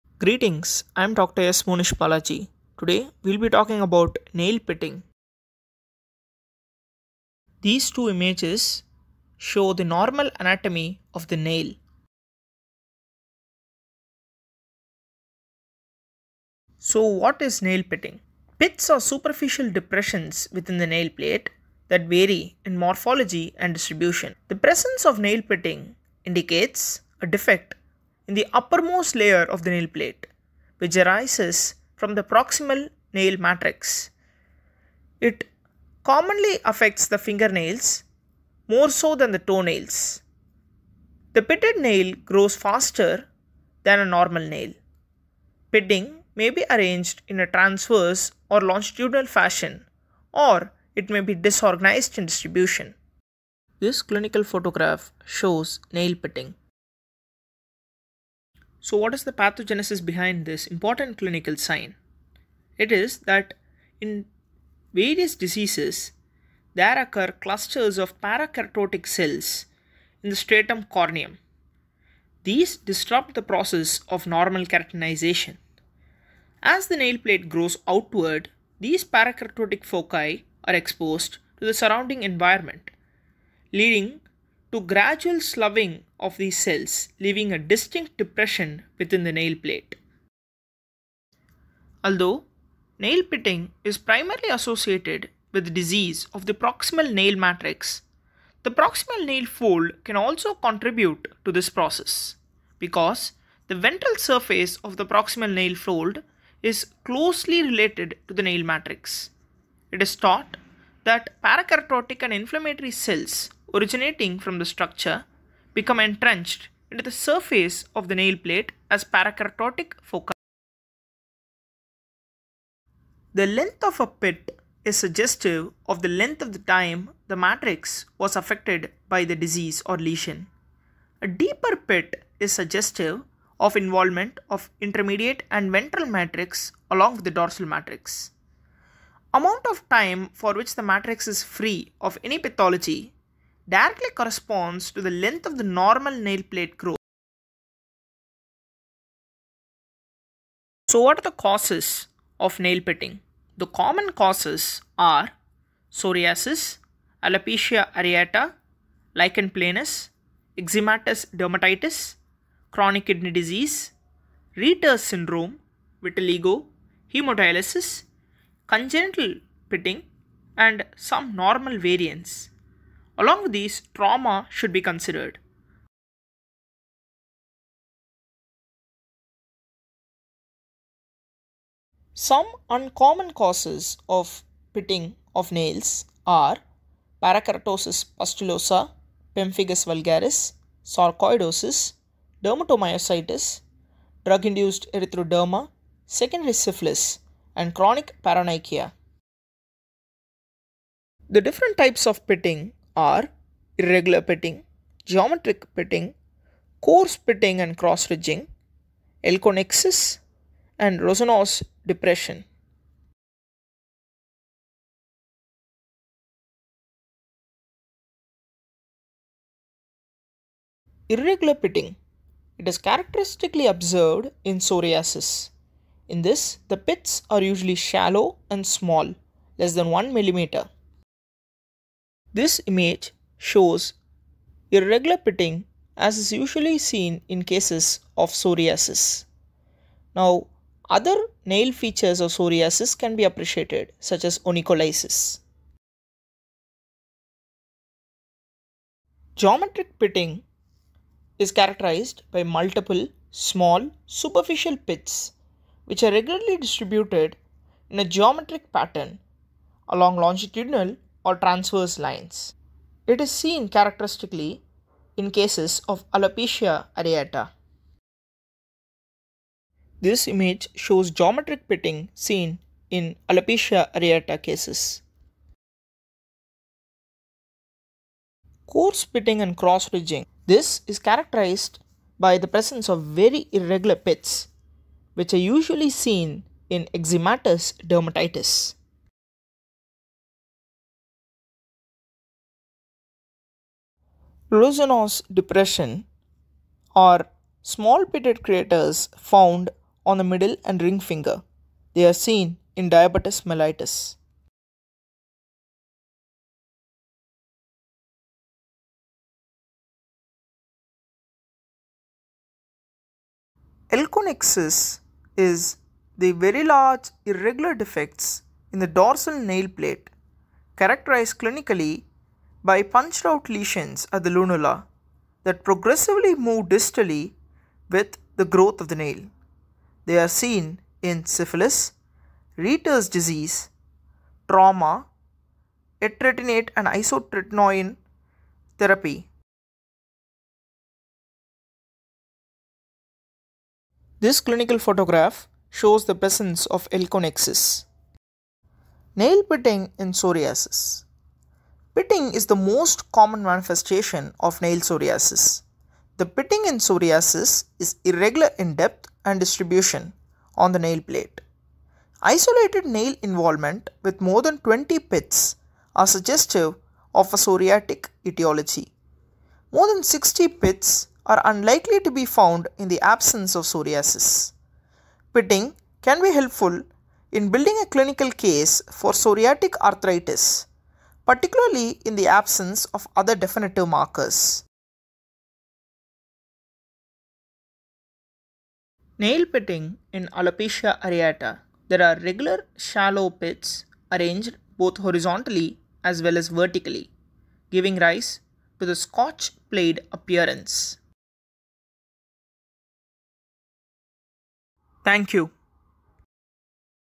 Pitting of Nails
This is a video on an important commonly observed clinical sign which may be due to various diseases. It deals with the pathogenesis types and etiology of this sign with additional information on it's characteristics when associated with psoriasis and alopecia areata. This topic is of importance in several fields including dermatology and rheumatology.